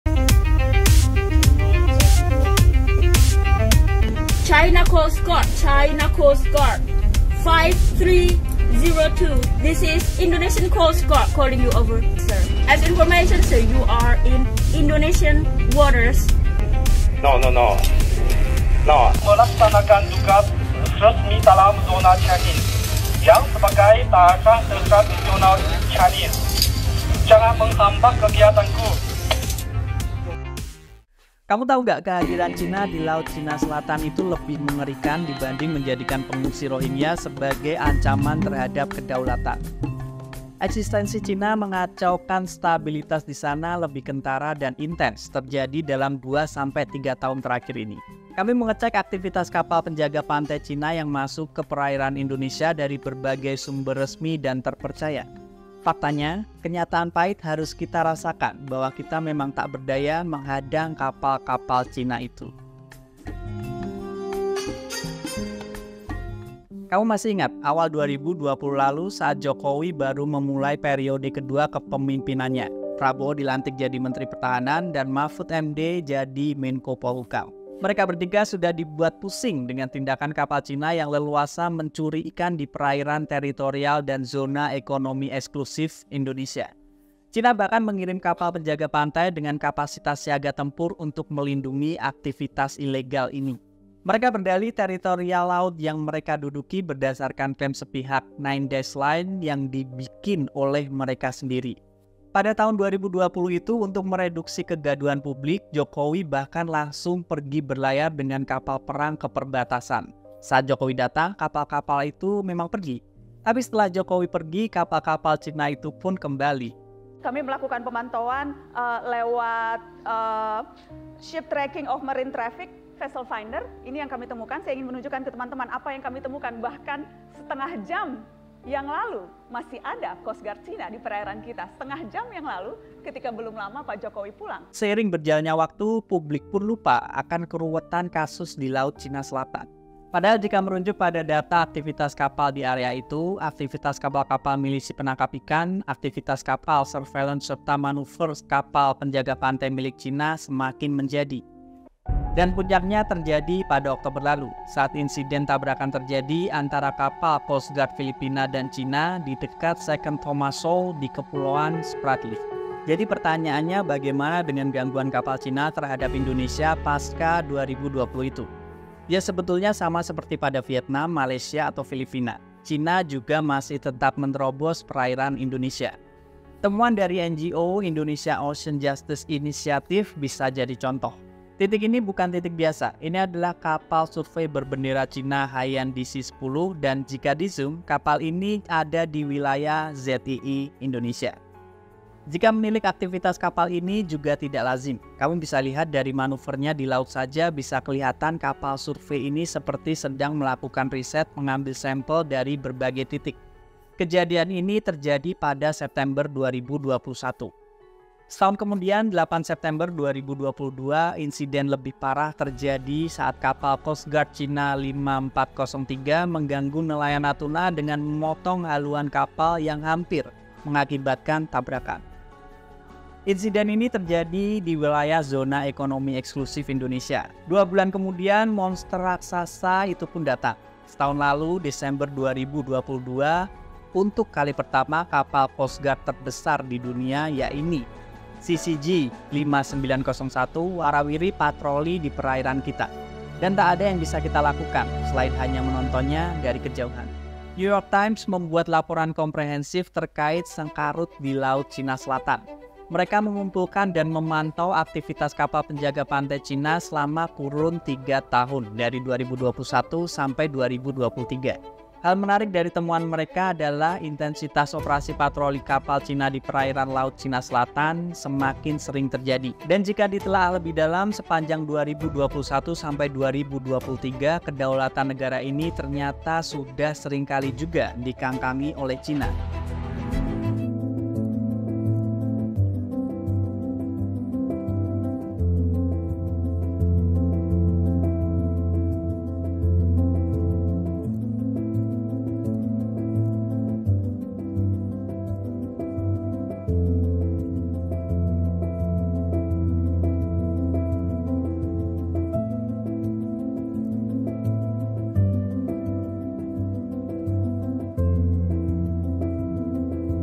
China Masih Bolak-balik di Natuna. Kita Cuma Diam Aja? | Buka Mata
Terkini, aktivitas kapal China di Laut China Selatan, khususnya yang berkaitan dengan Indonesia, semakin mengkhawatirkan. Temuan terbaru menunjukkan bahwa kapal-kapal China, termasuk kapal penjaga pantai dan milisi penangkap ikan, terus melanggar kedaulatan Indonesia dengan menerobos masuk ke perairan dan Zona Ekonomi Eksklusif (ZEE) Indonesia.

Pada September 2021, kapal survei China, Hai Yang Di Zhi 10, terdeteksi melakukan manuver yang tidak biasa di wilayah ZEE Indonesia, yang menunjukkan aktivitas pengambilan sampel atau riset. Satu tahun kemudian, pada September 2022, kapal penjaga pantai China 5403 terlibat dalam insiden yang mengganggu nelayan Natuna, hampir menyebabkan tabrakan.

Puncaknya adalah pada Desember 2022, ketika kapal penjaga pantai terbesar di dunia, CCG5901, terlihat berpatroli di perairan Indonesia, meningkatkan kekhawatiran tentang dominasi China di kawasan tersebut. Laporan komprehensif dari New York Times menyoroti intensitas operasi patroli kapal China di Laut China Selatan, yang semakin sering terjadi dari tahun 2021 hingga 2023, mengindikasikan pelanggaran berulang terhadap kedaulatan negara-negara di kawasan ini.

(Narasi)